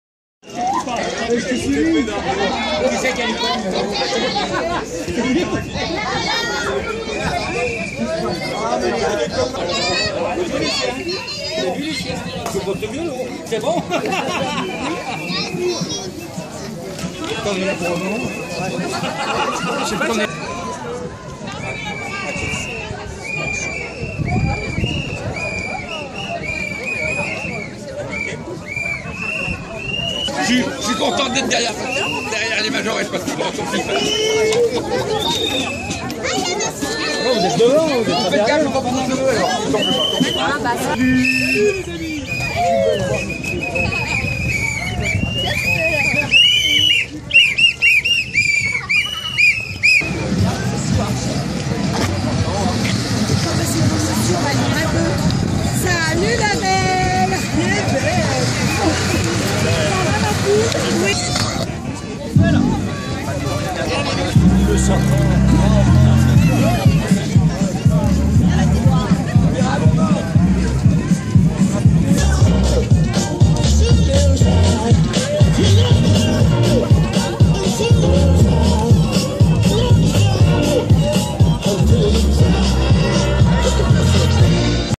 47ème CARNAVAL de SPC - Les chars
Vue de quelques chars mis en place juste avant le grand défilé pour le carnaval du 15 mars 2015.